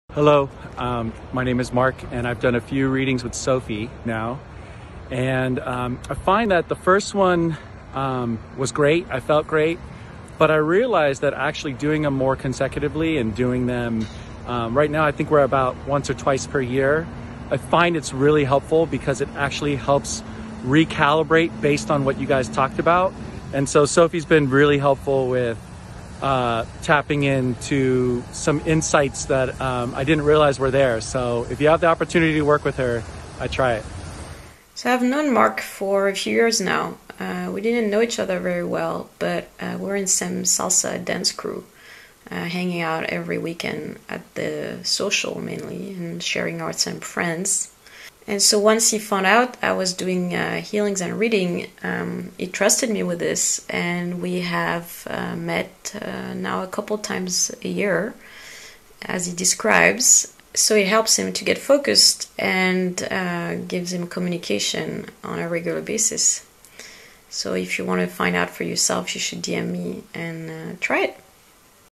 Reading sessions with Mark
Mark describes how he finds our sessions helpful, we meet a couple times a year, in person or remote.